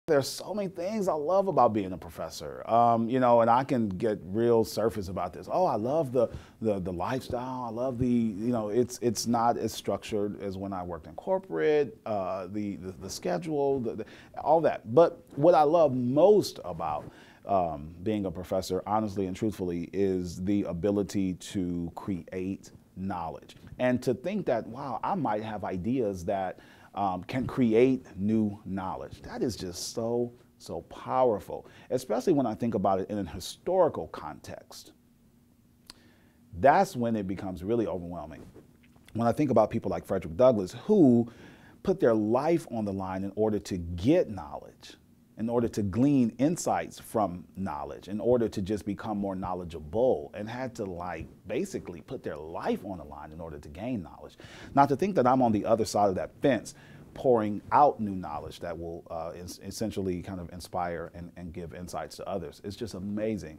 Why you can’t miss The PhD Project conference
Dr. Terry Esper reminds you why you want to become a professor. If you’ve been accepted to attend our conference, don’t pass it up! to learn more.